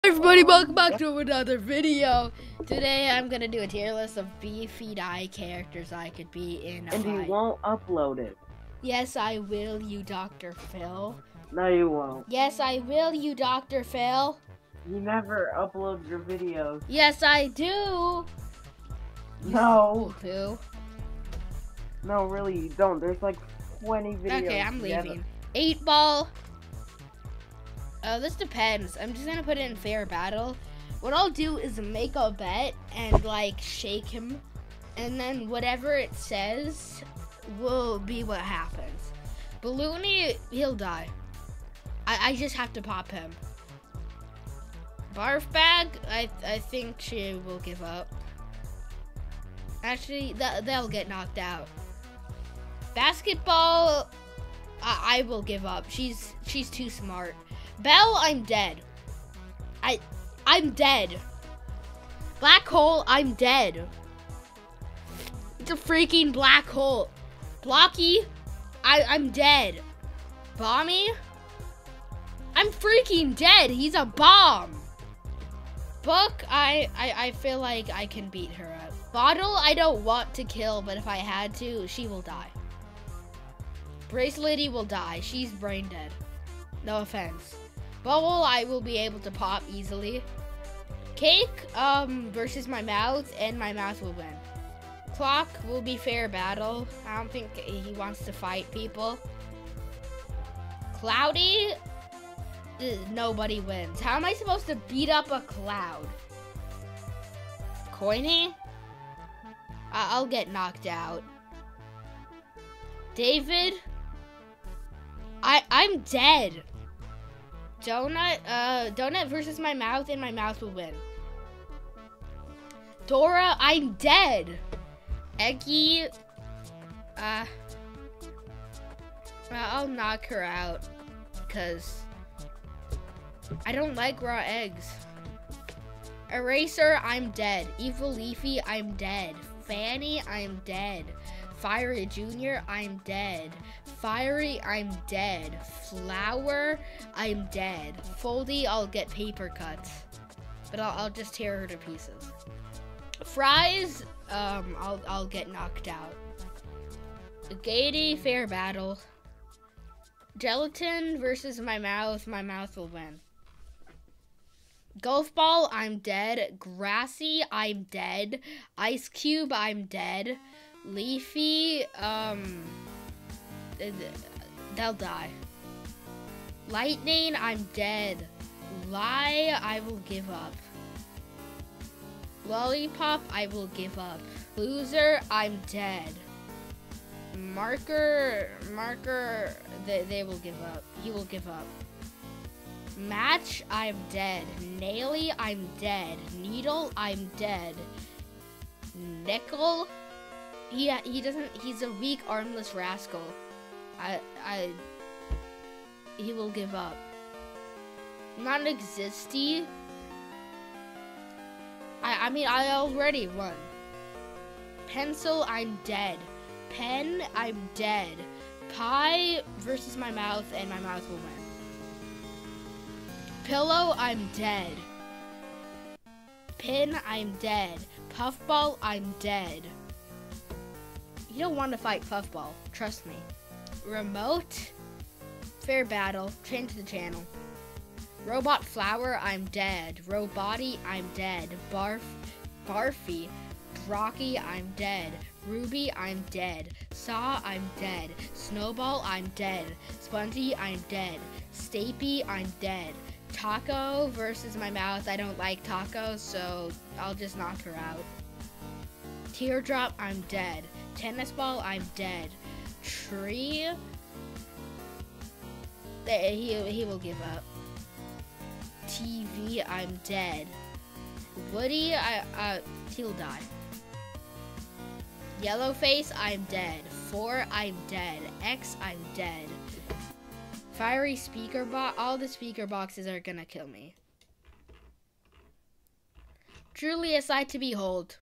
Ranking BFDI characters on how much I think I could beat them in a fight.
Ranking BFDI characters on how much I think I could beat them in a fight. A Bros Gaming